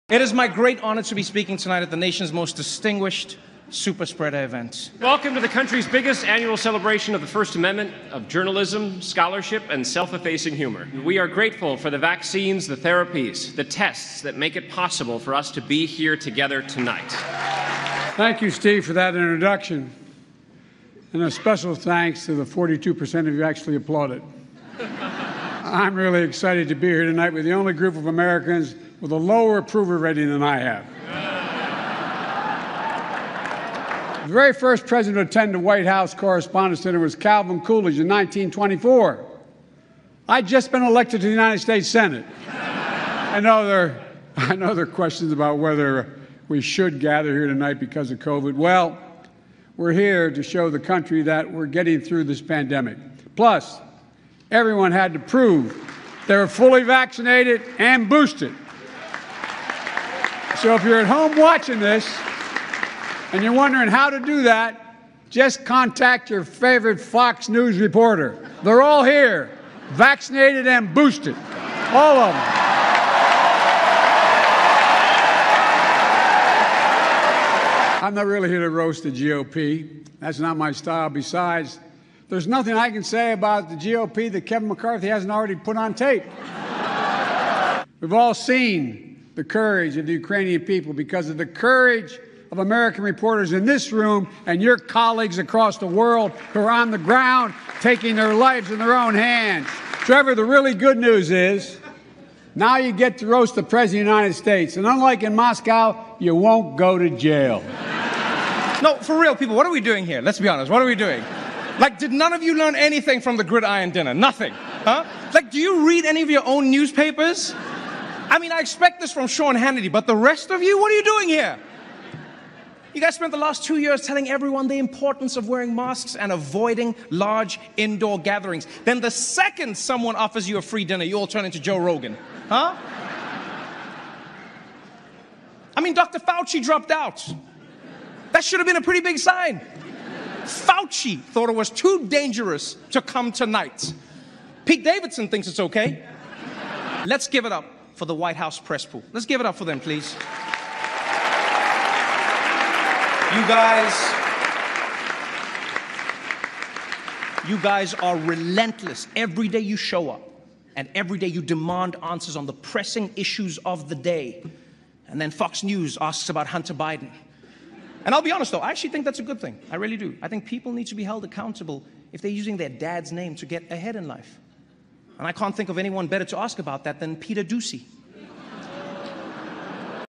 Trevor Noah's best jokes at the White House Correspondence Dinner April 30, 2022.
Trevor Noah's best jokes at the WHCD

The headliner quipped that it was his “great honor” to speak at the “nation’s most distinguished superspreader event.”

Here are 15 of Noah’s best lines.

It is my great honor to be speaking tonight at the nation’s most distinguished superspreader event. Did none of you learn anything from the Gridiron dinner? … The second someone offers you a free dinner you all turn into Joe Rogan.
I’m not doing this just for the attention. All right? I’m a comedian, not Kyrsten Sinema. … By the way, give it up for Kyrsten Sinema. Whoever thought we’d see the day in American politics when a senator could be openly bisexual, but closeted Republican? That’s progress.
We all saw what happened at the Oscars. I’ve actually been a bit worried about tonight. I’m not going to lie. What if I make a really mean joke about Kellyanne Conway, and her husband rushes up on the stage and thanks me?
I’ve just got to say, this is so exciting. To be at this swanky party full of Washington’s most powerful people. It’s not as exciting as Madison Cawthorn made it sound, but it’s still very sexy.
Trump said he won the election, but everyone was just able to look at the numbers and see that he was wrong. That’s why Ron DeSantis is one step ahead — first you ban the math textbooks, then nobody knows how to count the votes. Boom. My man!
The great chef José Andrés is here tonight. … Whenever there’s a disaster anywhere in the world, chef José is there, which I guess is why he’s sitting at the CNN table tonight.
The real reason that it’s such an honor to be here tonight is that we all get to be in the same room as the most powerful man in the United States. So let’s give it up for Joe Manchin, everybody.
Even as first lady, Dr. Biden continued her teaching career. The first time a presidential spouse has done so, ever. Congratulations. Now, you might think it’s because she loves teaching so much, but it’s actually because she’s still paying off her student debt. I’m sorry about that, Jill. I guess you should’ve voted for Bernie.
I think everyone will agree that it’s actually nice to once again have a president who’s not afraid to come to the White House Correspondents’ Dinner, and hear jokes about himself. I’ll be honest, if you didn’t come, I totally would have understood because these people have been so hard on you, which I don’t get. I really don’t. And I think ever since you’ve come into office, things are really looking up. You know, gas is up, rent is up, food is up, everything.
President Biden’s lack of a filter does get him into hot water sometimes. Last month, he caused a huge international incident saying that Vladimir Putin should be removed from power. It was very, very upsetting to Russia until someone explained to them that none of the stuff Biden wants actually gets done.
What about Maggie Haberman? For four years, it was exclusives on the Russia investigation, corruption, the president doesn’t read his daily briefings, on and on. Now look at her. She spends all day fighting with random people on Twitter like a common POLITICO reporter. You’ve ruined her Mr. President.
Jen [Psaki], it’s nice that you’re willing to come over here and risk getting Covid for like, what, the 10th time now? Let me ask, how do you keep getting Covid, Jen? Like your boss hooked us all up with free tests. Does he not have your address? What’s going on there?
I’m really excited because the kings of cable are here. Fox News in the house … I know Fox has a bad reputation — I can even feel you tense up now when I talk about them. They really do crush it. You know, I think they get a bad rap. Because it’s a mixed bag. They actually have really good journalists, it just depends on when you watch. Fox News is sort of like a Waffle House. Yeah, it’s relatively normal in the afternoon, but as soon as the sun goes down, there’s a drunk lady named Jeanine threatening to fight every Mexican who comes in.
Apparently, Jeff [Zucker] got fired after he tried to keep his workplace relationship secret, which is weird because if he really didn’t want anyone to know about it, he could have just made a show about it on CNN+. … It’s so sad. CNN+, gone but forgotten. You know who I blame? You know who I blame, CNN? John King … your magic wall can predict how every person in the country is going to vote in every county, but it couldn’t give you a heads up that nobody wanted more CNN?
Please be careful leaving tonight, we all know this administration doesn’t handle evacuations well.